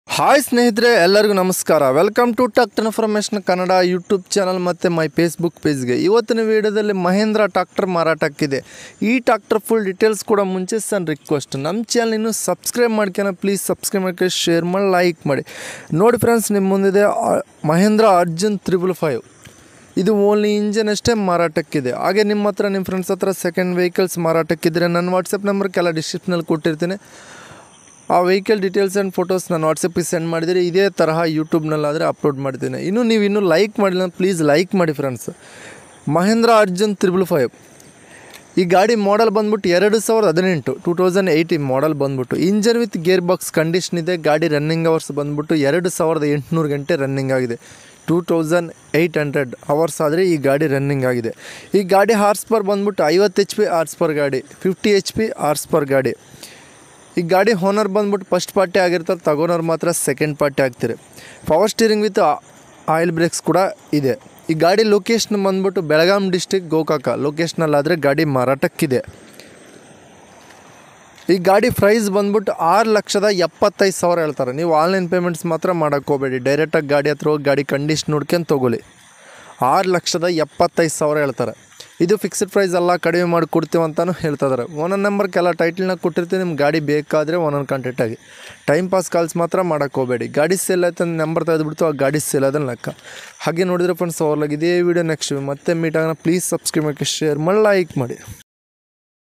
Mahindra 555 DI for slae 8152092116 second hand used tractor sale in Karnataka
Mahindra 555 DI for slae  second hand used tractor sale in Karnataka

owner number...8152092116